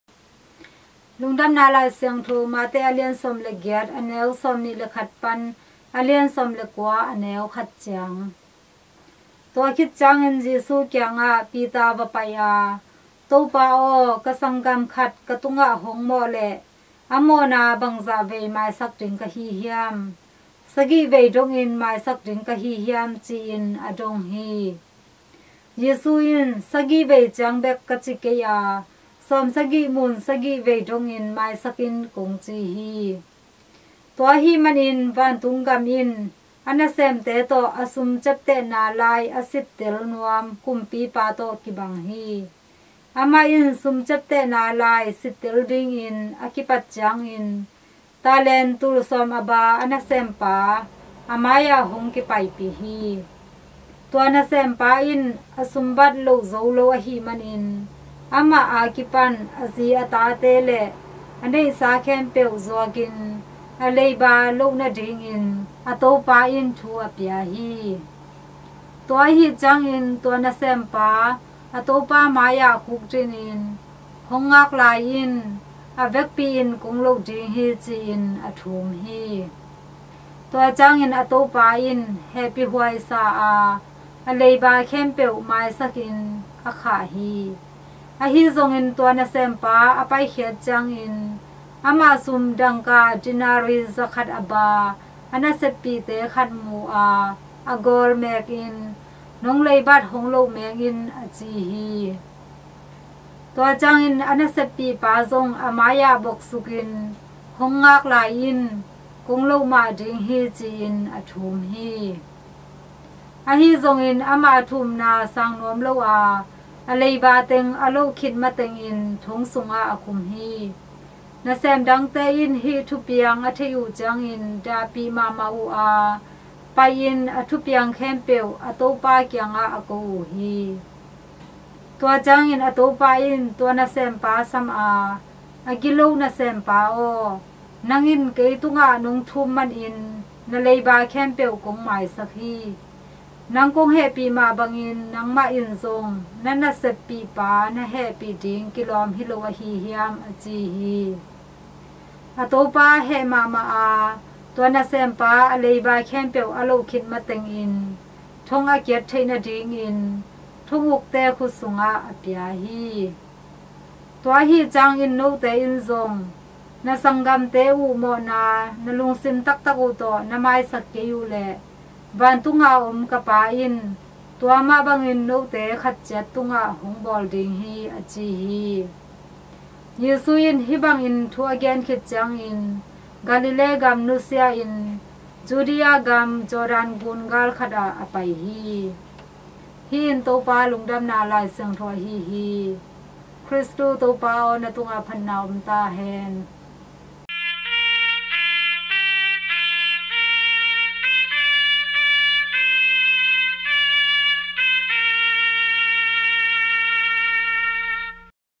Catholic Nisim Lungdamna Laisiangtho. Hun Nautaang Nipi Kaal (19) na. 15.8.2024 (Thursday)
Lungdamna Laisiangtho  Mt 18;21-19;1 simna